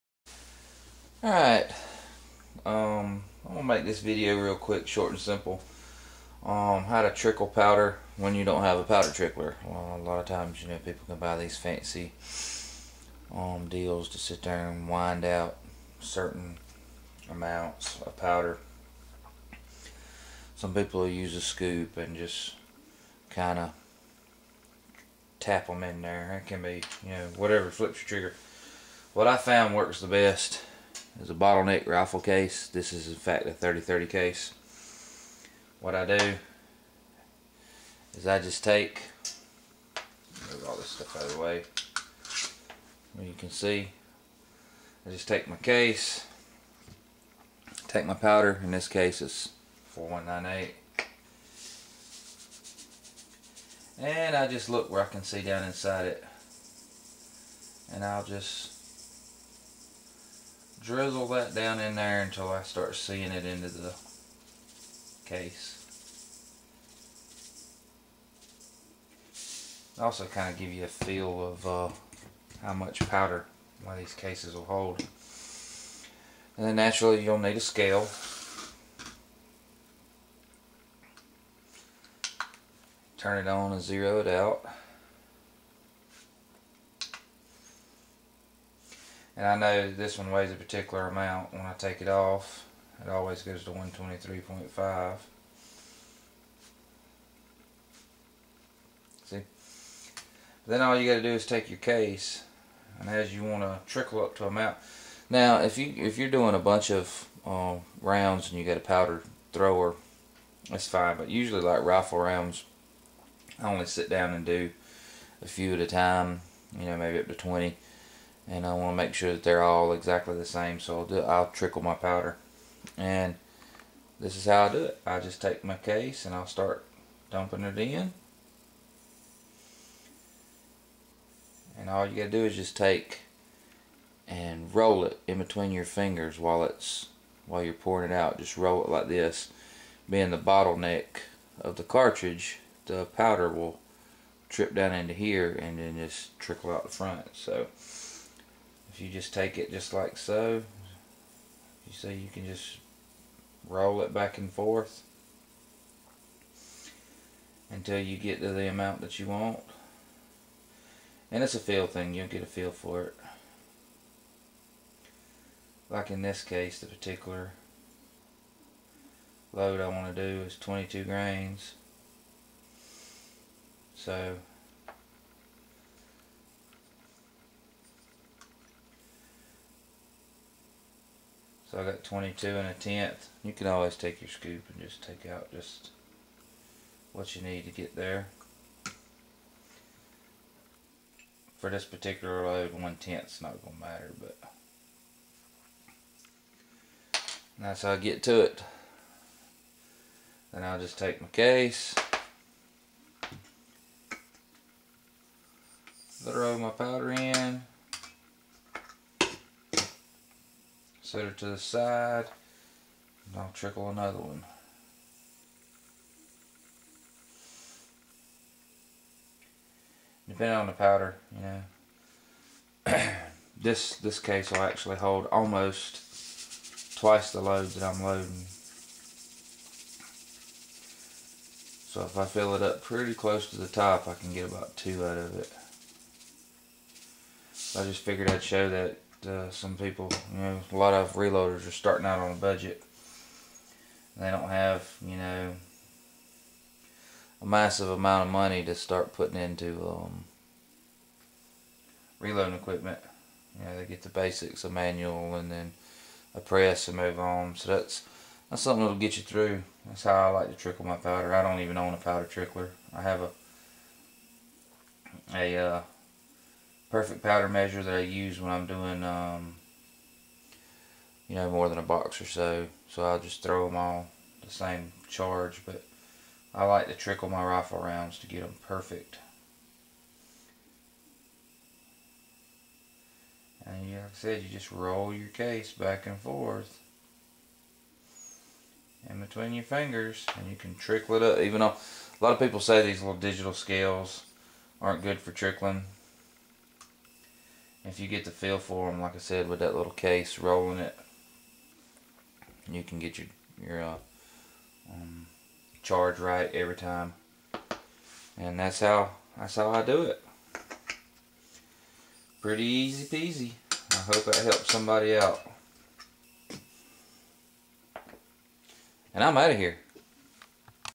Poor man's powder trickler.
Here is an easy way to trickle powder without any money invested.